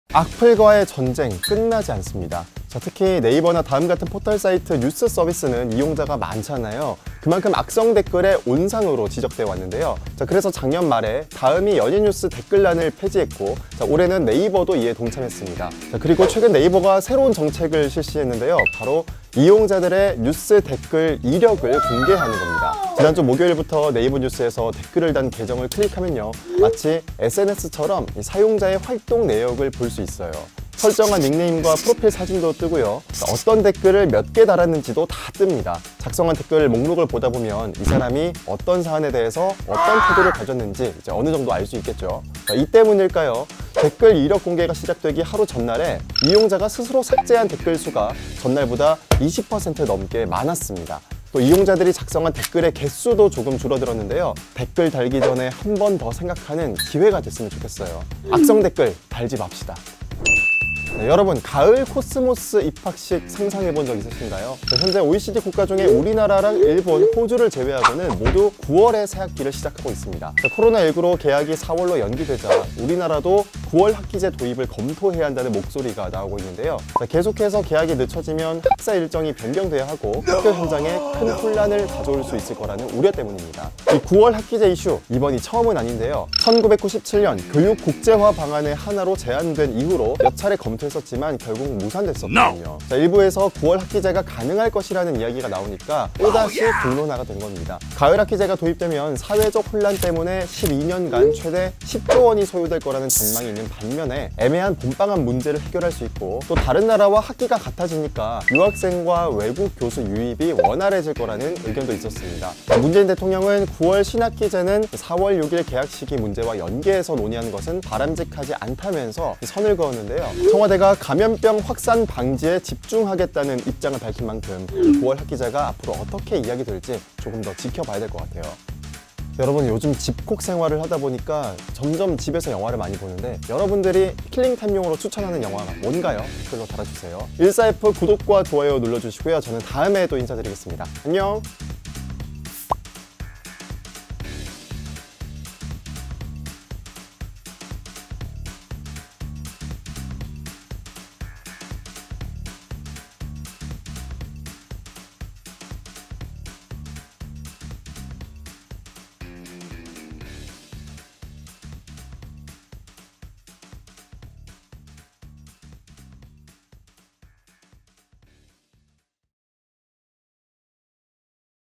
네이버가 뉴스기사 댓글 이력 공개하자 생겨난 변화(?) / 14F
네이버가 지난 19일부터 뉴스서비스에 댓글을 단 이용자의 프로필 사진, 닉네임, 작성한 댓글 등을 공개하고 있습니다. 이전까지는 모두 비공개로 설정할 수 있어 악성댓글의 온상으로 지목되기도 했는데요. 이 때문인지, 이용자들의 댓글 자진삭제가 늘어났다고 합니다. 이번 규제가 악성댓글을 줄일 수 있을까요?